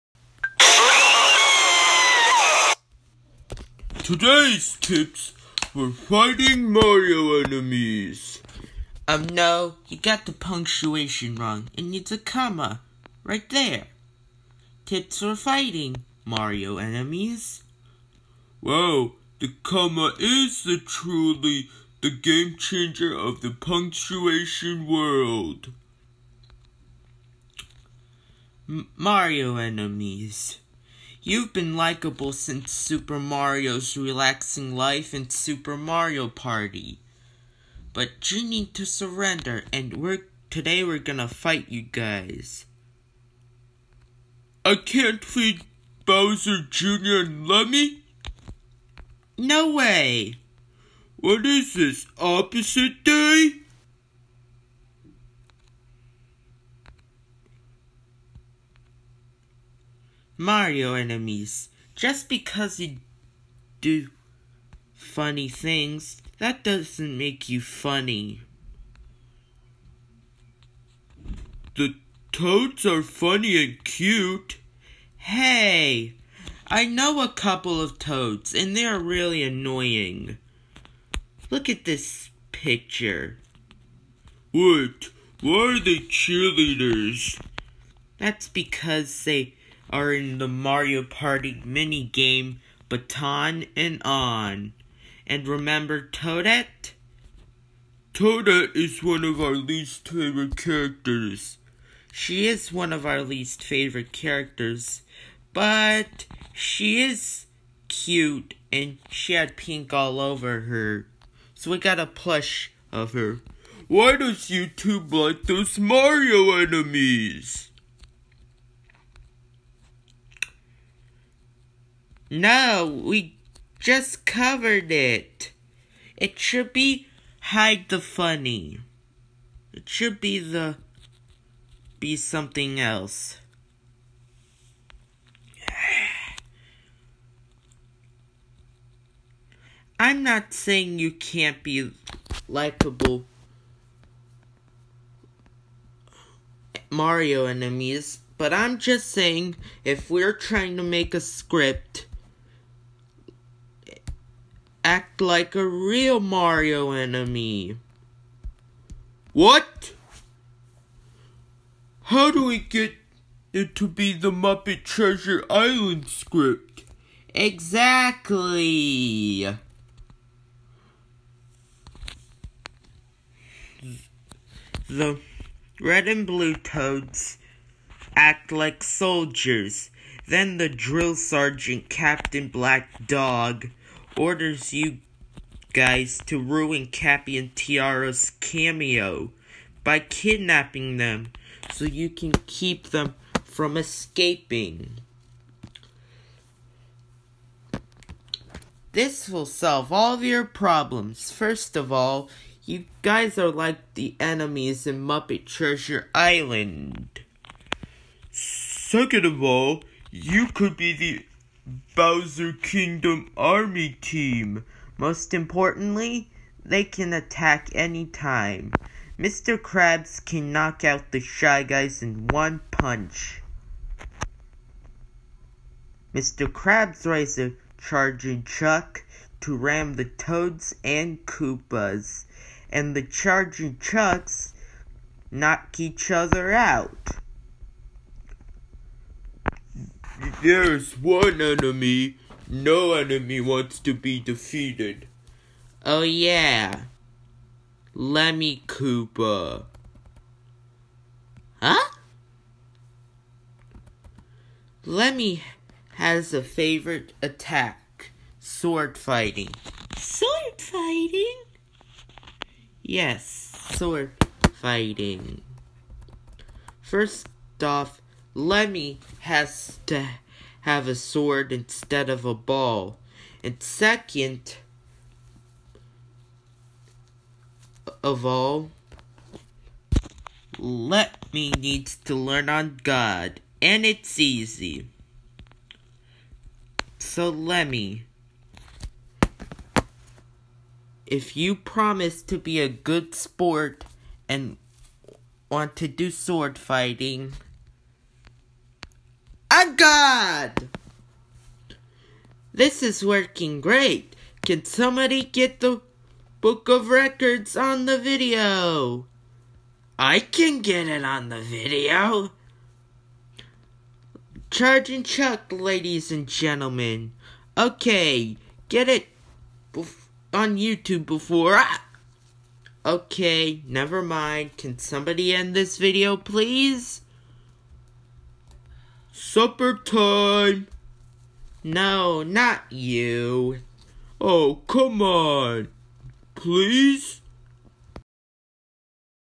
Tips for Fighting, Mario Enemies!
If someone is trying to like you act like a real Mario enemy.